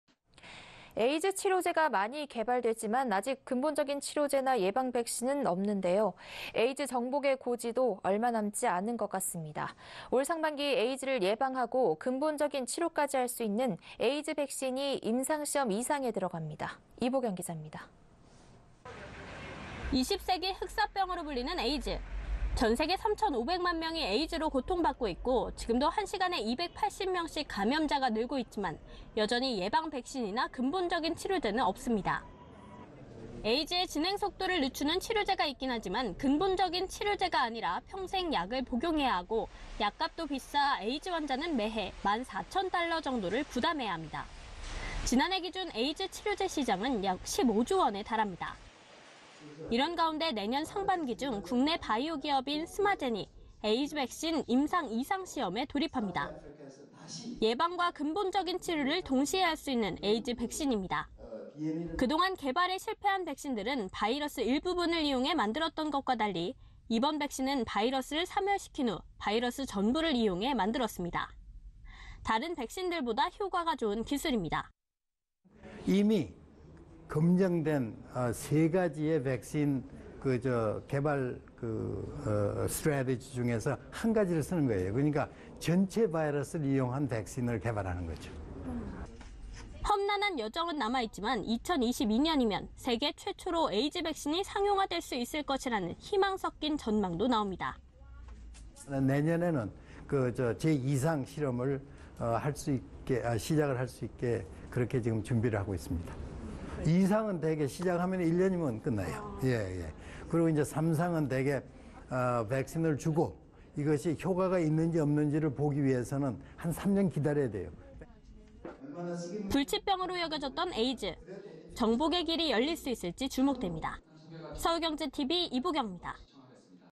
[서울경제TV] 에이즈 백신 나오나… 내년 상반기 임상 2상 돌입
[앵커]
에이즈,  치료제가 많이 개발됐지만 아직 근본적인 치료제나 예방 백신은 없는데요. 에이즈 정복의 고지도 얼마 남지 않은 것 같습니다. 올 상반기 에이즈를 예방하고 근본적인 치료까지 할 수 있는 에이즈 백신이 임상시험 2상에 들어갑니다. 이보경기잡니다.

[기자]
20세기 흑사병으로 불리는 에이즈.
전세계 3,500만명이 에이즈로 고통받고 있고 지금도 한시간에 280명씩 감염자가 늘고 있지만 여전히 예방백신이나 근본적인 치료제는 없습니다.
에이즈의 진행속도를 늦추는 치료제가 있긴 하지만 근본적인 치료제가 아니라 평생 약을 복용해야 하고 약값도 비싸 에이즈 환자는 매해 1만4,000달러 정도를 부담해야 합니다.
지난해 기준 에이즈 치료제 시장은 약 15조원에 달합니다.

이런 가운데 내년 상반기 중 국내 바이오기업 ‘스마젠’이 에이즈백신 임상 2상 시험에 돌입합니다.
예방과 근본적인 치료를 동시에 할 수 있는 에이즈 백신입니다.

그동안 개발에 실패한 백신들은 바이러스 일부분을 이용해 만들었던 것과 달리 이번 백신은 바이러스를 사멸시킨 후 바이러스 전부를 이용해 만들었습니다.
다른 백신들보다 효과가 좋은 기술입니다.

[인터뷰] 강칠용 / 웨스턴온타리오 대학 의과대학 교수 4분
이미 검증된 세 가지 백신 전략 중 하나를 쓰는거에요. 전체 바이러스를 이용한 백신을 개발하는 거죠.

험난한 여정은 남아있지만 2022년이면 세계 최초로 에이즈 백신이 상용화 될 수 있을 것이라는 희망 섞인 전망도 나옵니다.

[인터뷰] 강칠용 / 웨스턴온타리오 대학 의과대학 교수
내년에는 2상 실험을 시작할 수 있게 준비하고 있습니다. 2상은 대개 1년이면 끝나요. 3상은 대개 백신을 주고 효과가 있는지 없는지를 보기 위해서는 한 3년 기다려야되요.

불치병으로 여겨졌던 에이즈, 정복의 길이 열릴수 있을지 주목됩니다.